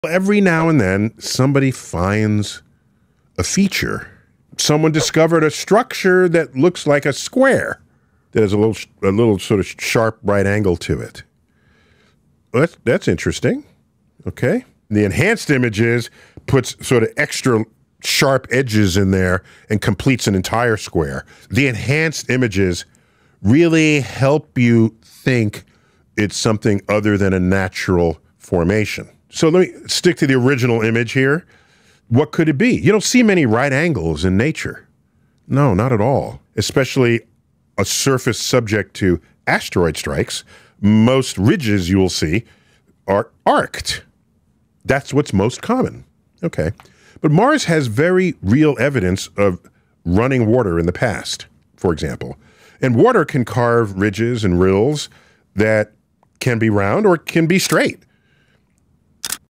Neil deGrasse Tyson on Alien Faces on the Moon
Is there life on the Moon? Neil deGrasse Tyson dives into the mysterious sightings of strange structures, faces, and geometrical patterns reported in various Moon images and videos. He discusses the science behind these illusions, the psychology of pareidolia, and whether alien civilizations could be involved. This video explores theories behind alien life, Moon anomalies, and UFO sightings, while staying rooted in scientific reasoning. If you've ever wondered about what's really on the Moon—this is for you.